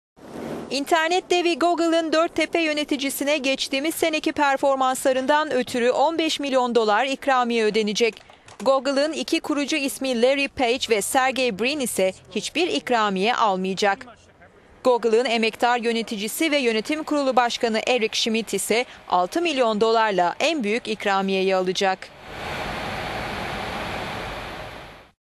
Google'dan üst düzey yöneticilerine 15 milyon dolar ikramiye
İnternet devi Google'ın dört tepe yöneticisine, geçtiğimiz seneki performanslarından ötürü 15 milyon dolar ikramiye ödeyecek. Google'ın iki kurucu ismi Larry Page ve Sergey Brin ise hiçbir ikramiye almayacak.

Google'ın emektar yöneticisi ve yönetim kurulu başkanı Eric Schmidt, 6 milyon dolarla en yüksek ikramiyeyi alacak.